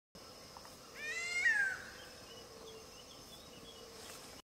Gray headed Kite call
Gray headed Kite call  (@Wildtalk)
The gray-headed kite is a raptor found in open woodland and swamp forests. It shares the genus Leptodon with the extremely rare white-collared kite. It breeds from eastern Mexico and Trinidad south to Peru, Bolivia, Brazil, and northern Argentina